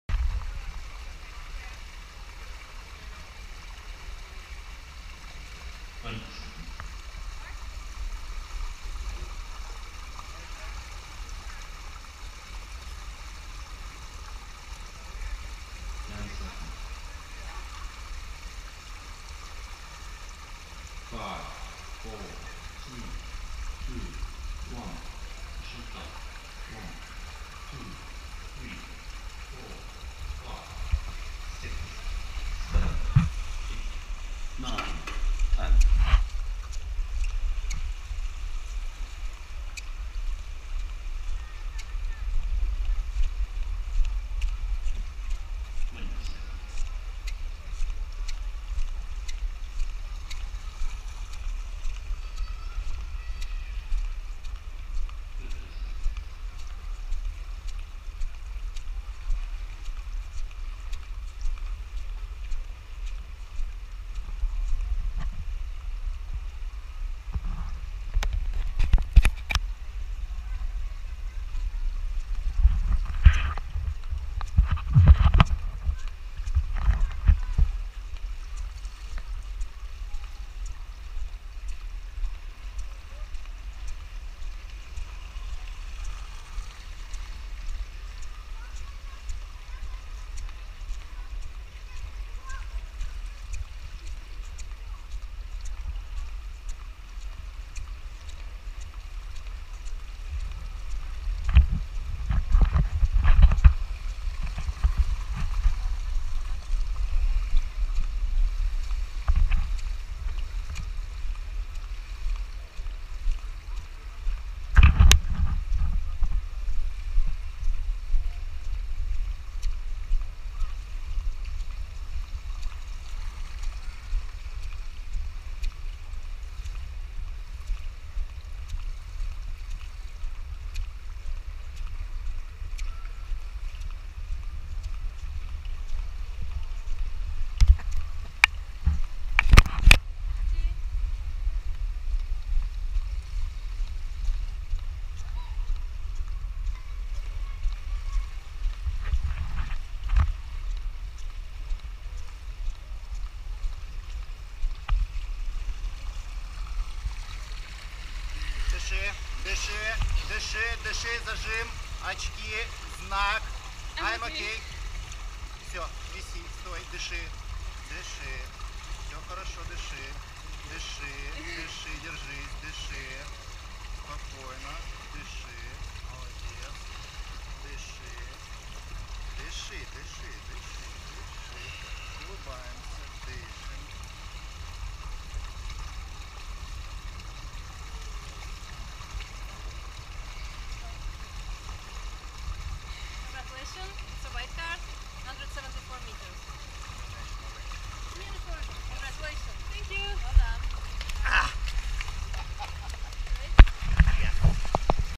Sadurska Kateryna Ukrainian Freediving National Record DYN 174m
Ukrainian Freediving National Record DYN 174m
XIV Basenowe Mistrzostwa Polski we Freedivingu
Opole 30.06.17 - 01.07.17

Открытый чемпионат польши по фридайвингу.
Екатерина Садурская
Национальный рекорд Украины в дисциплине динамика в ластах 174 м

Съемка Денис Рылов.